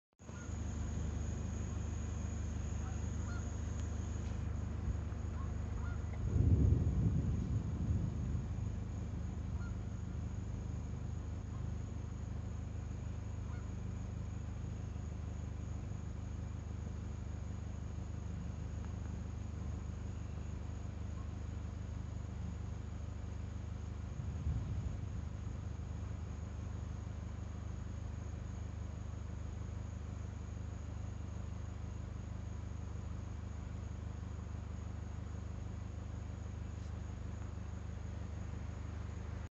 Late Evening Thunderstorm [Part 1]
recorded in Northwest GA, facing South